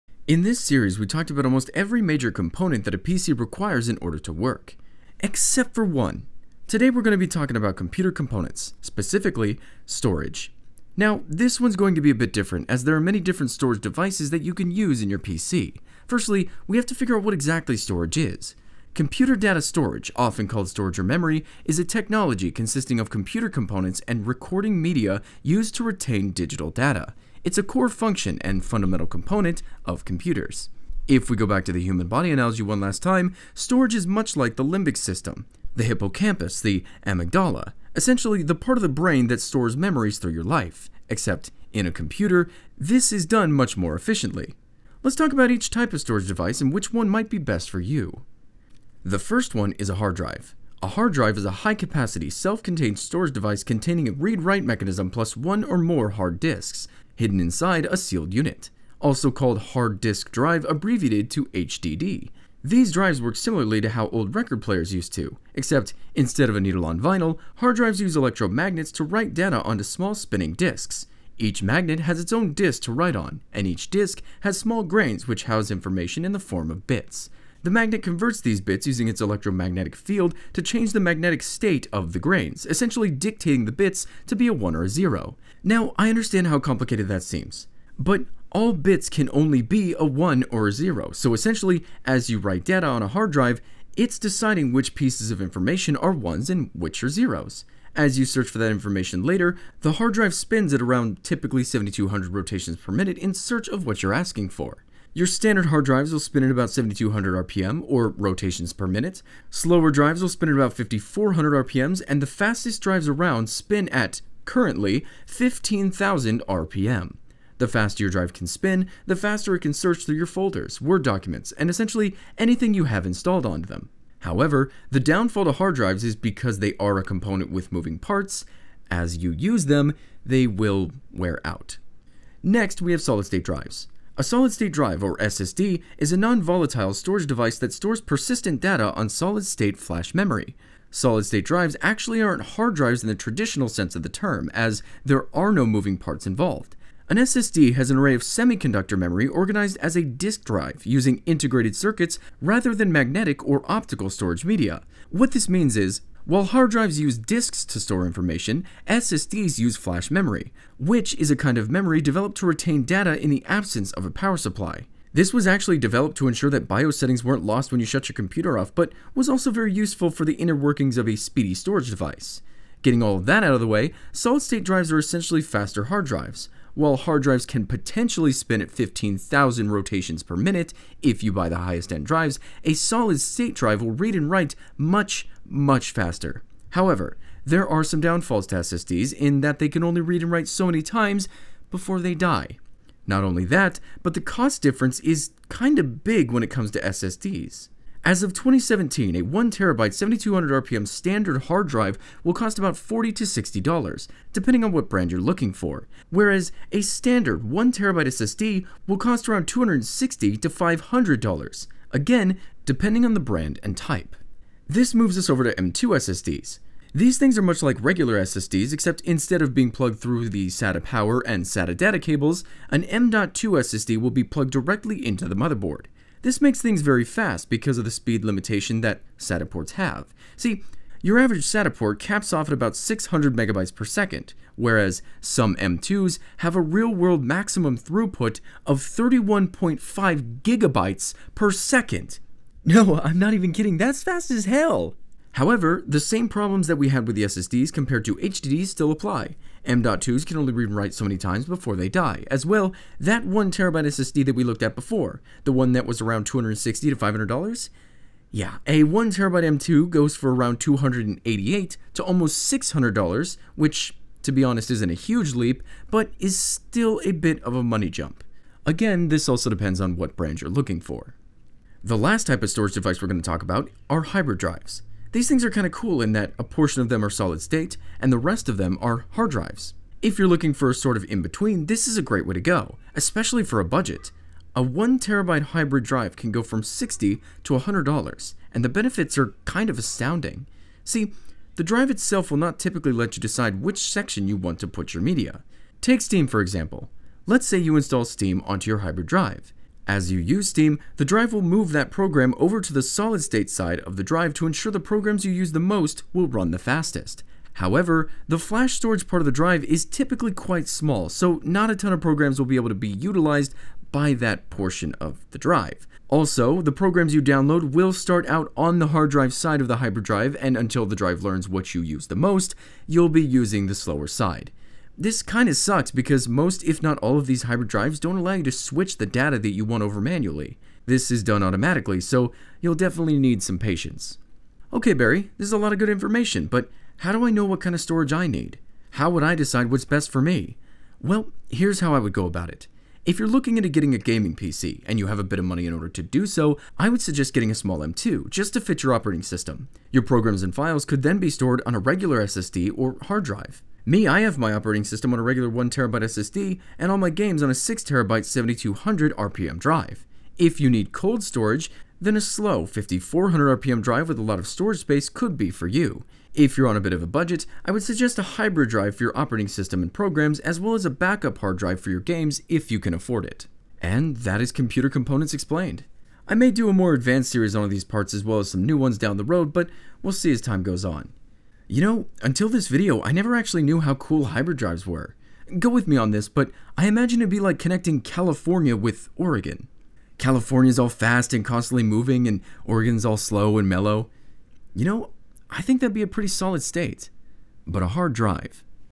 Computer Components Explained: Storage
Today we are going to talk about storage, this video should teach you all you need to know about hard drives, ssd's, m.2 ssd's and hybrid drives! I hope you enjoy!

White City Oregon
97503